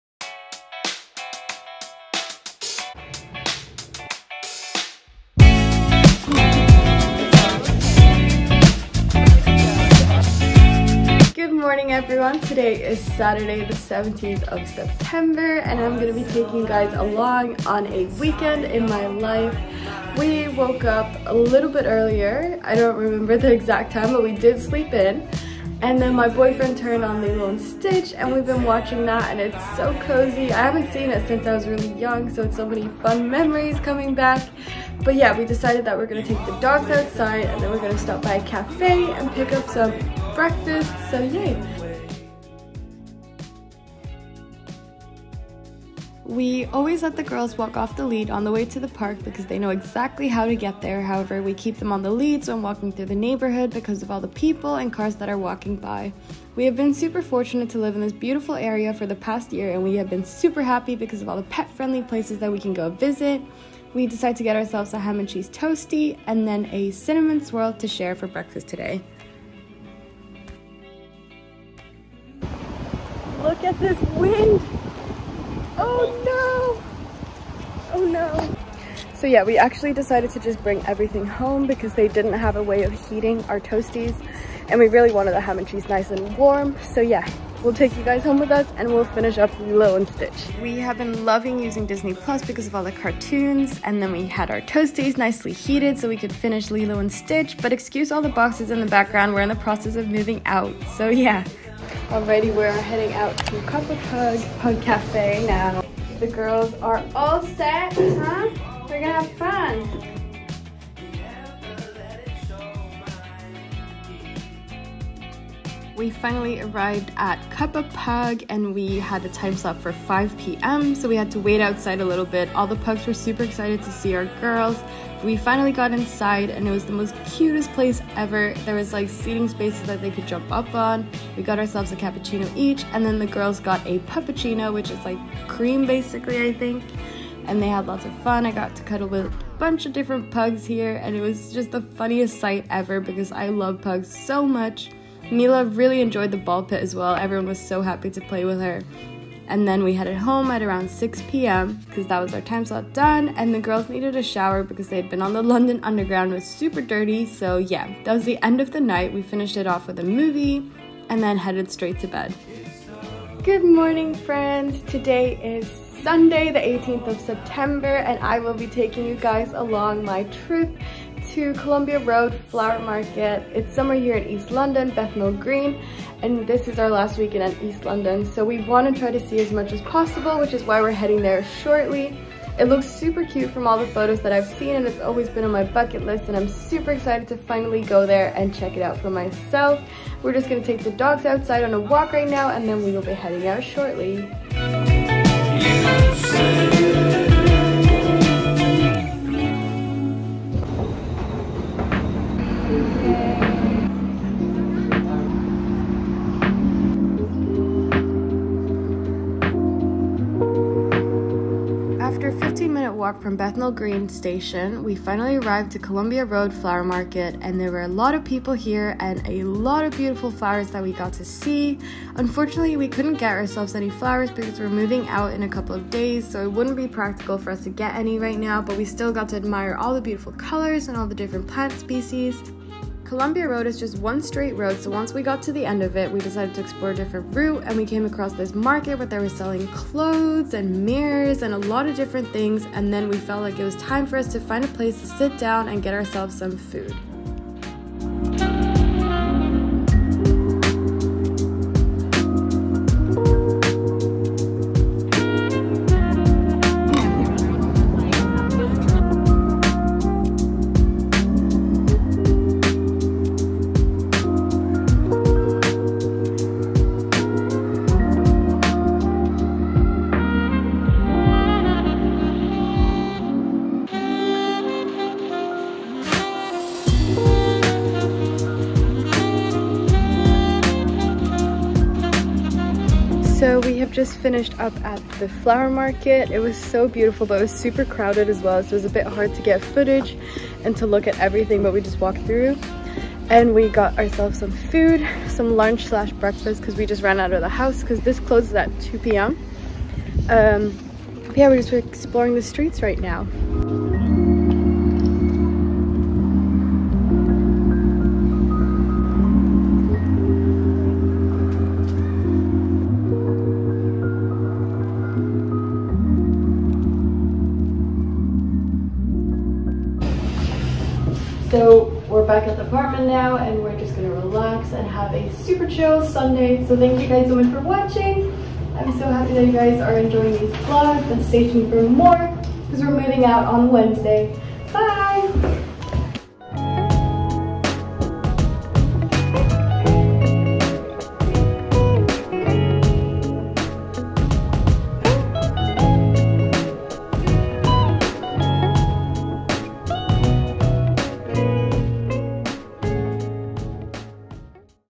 Weekend Vlog London | Colombia Road Flower Market, Pug Cafe, Brunch & more✨
Hello friends! 💗

I tried a little bit a different editing style for this vlog with voiceovers and I hope you enjoyed it. This weekend was all about spending quality time in the East London area that we are currently living in as this was our last weekend here. We explore pet friendly cafes, went to the pug cafe and Colombia road flower market.